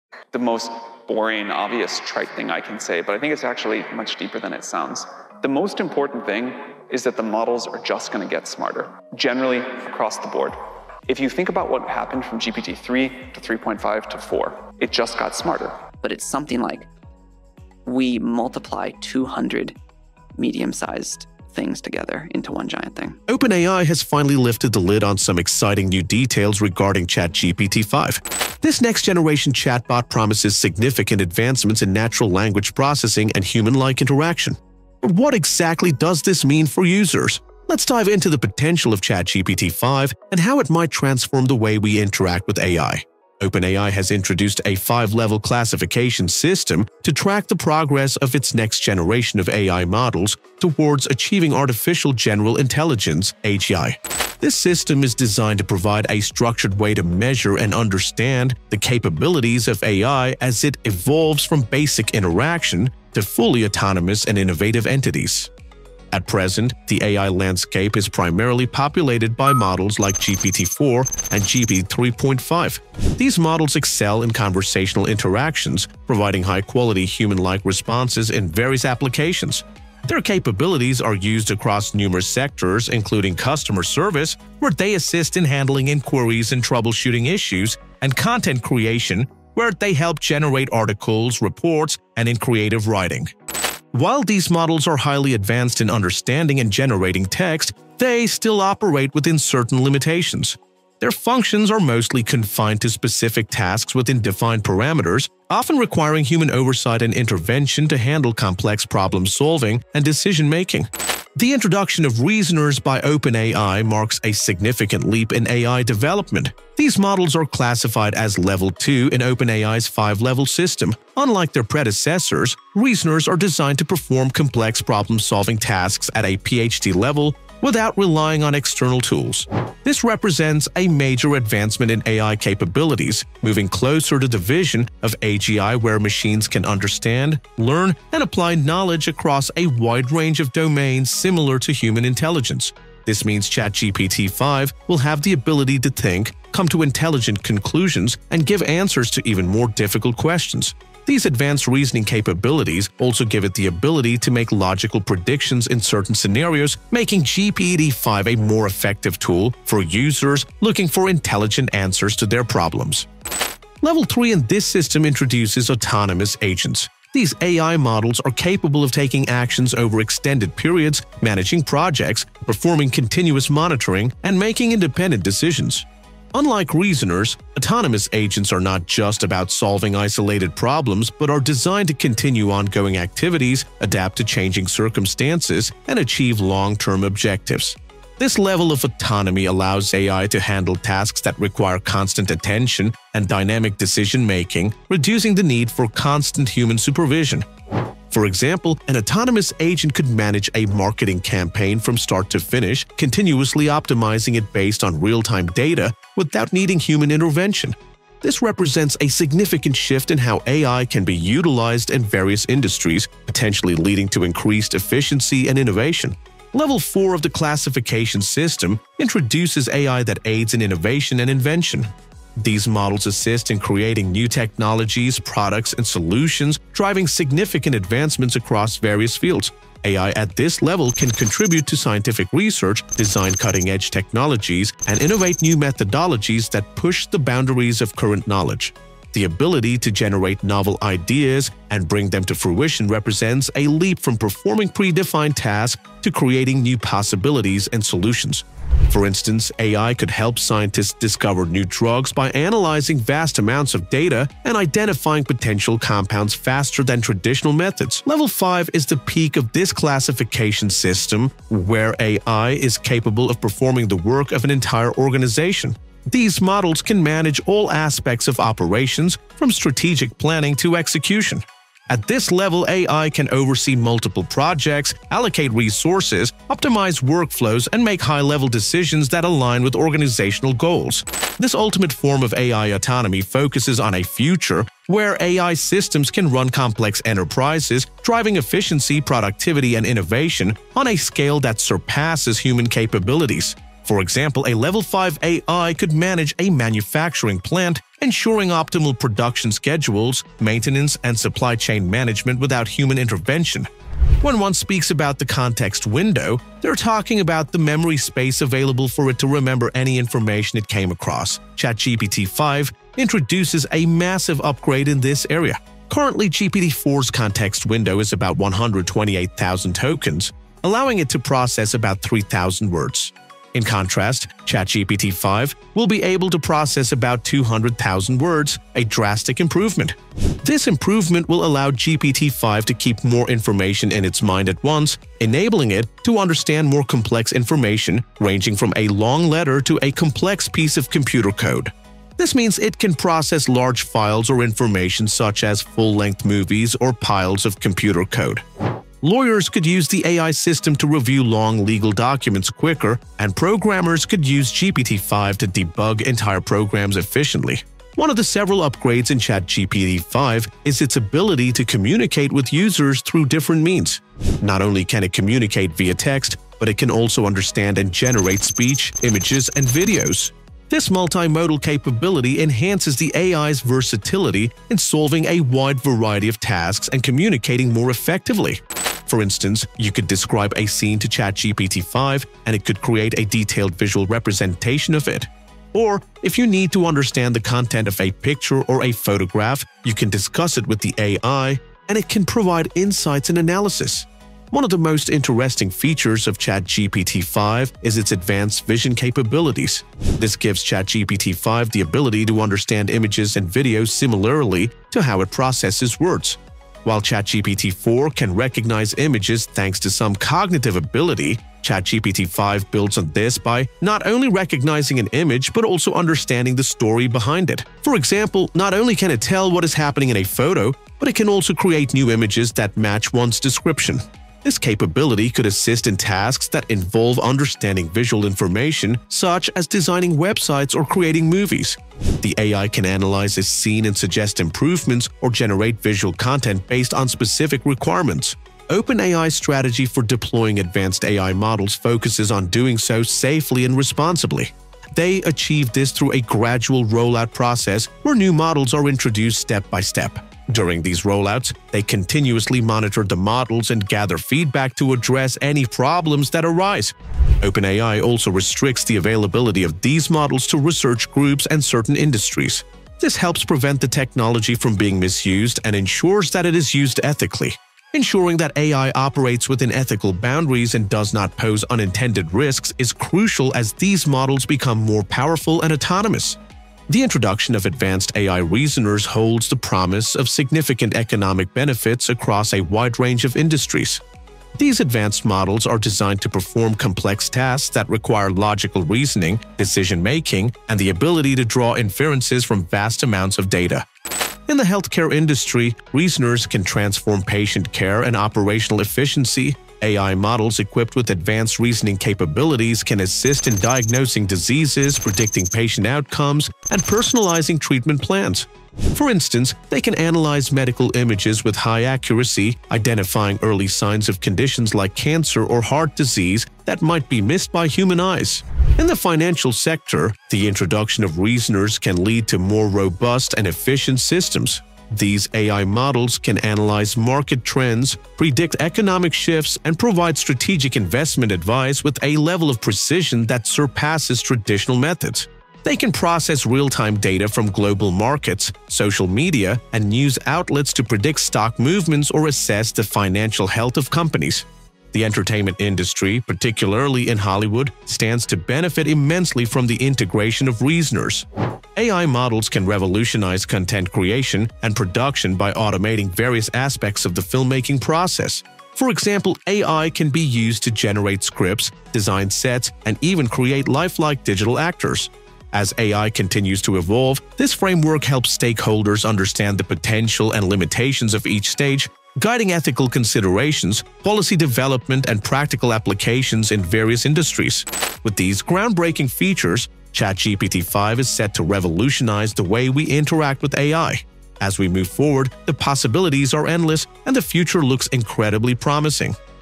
New ChatGPT-5 Details Revealed By Open AI!
OpenAI has finally revealed exciting new details about ChatGPT-5, showcasing significant advancements in natural language processing and human-like interaction. This next-generation chatbot promises to transform the way we interact with AI, offering unprecedented reasoning capabilities, extensive context retention, and multimodal communication. Also we dive into the potential of ChatGPT-5, exploring how it might revolutionize various industries, enhance user experiences, and move us closer to achieving artificial general intelligence (AGI). Don't miss out on discovering how these groundbreaking features could reshape the future of AI!